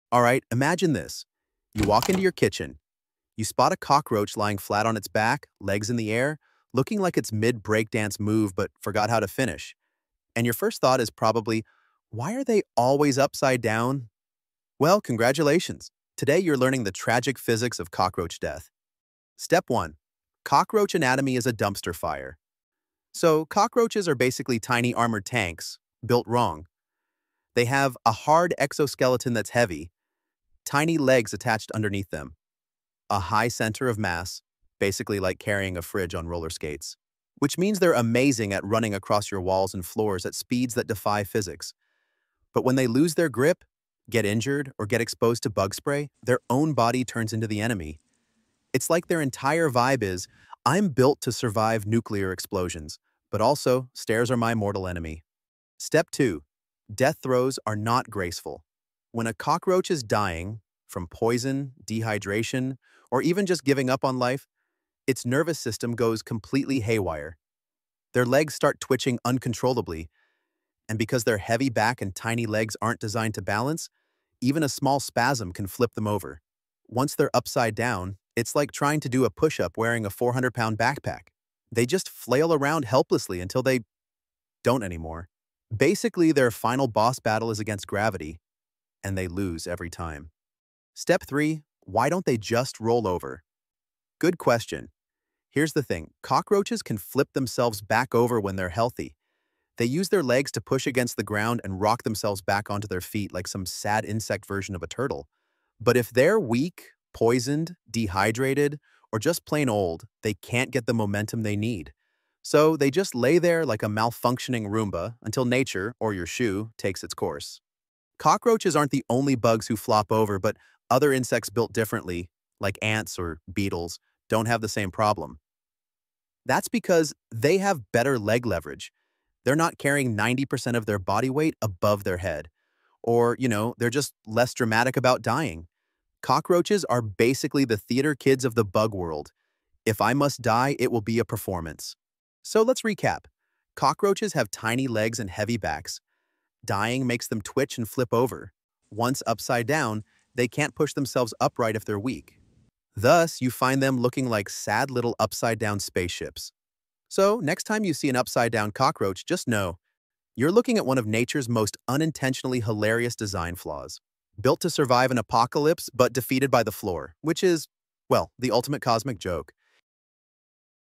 Why Do Cockroaches Always Die Upside Down
Ever noticed how cockroaches always seem to die on their backs? 🤔 In this video, we dive into the strange science behind this creepy phenomenon. From their unique anatomy to how insecticides affect their nervous systems, we break down the real reasons these resilient bugs end up belly-up.

🔬 What you'll learn:

The role of cockroach physiology and center of gravity

How pesticides disrupt their coordination

Why they struggle to flip back once they're upside down

Bonus: Is it always a sign they’re dead?